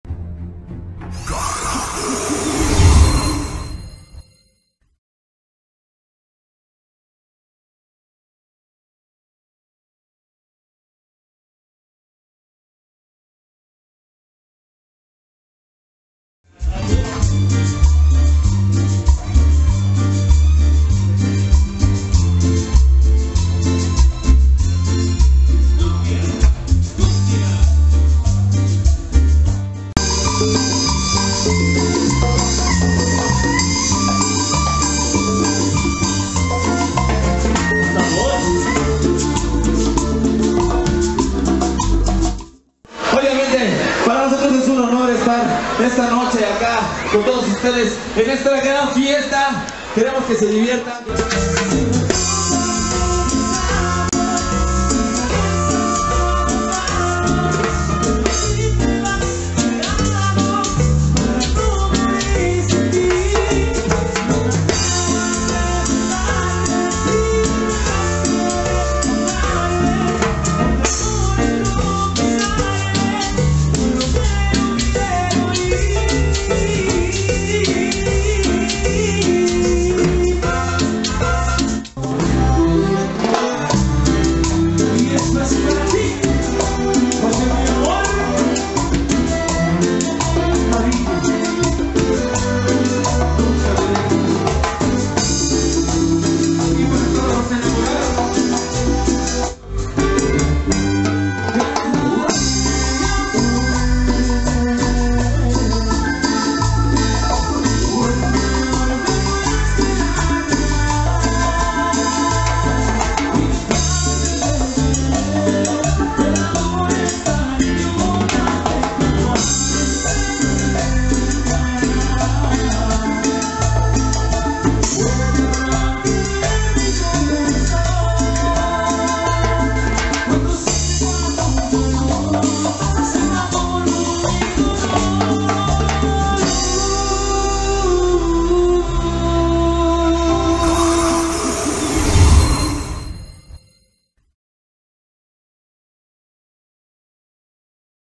GRUPO CARABO 2016 en Ignacio Allende Huixquilucan
Presentación de Grupo Carabo en la Fiesta patronal de Ignacio Allende Huixquilucan, 15 de mayo 2016.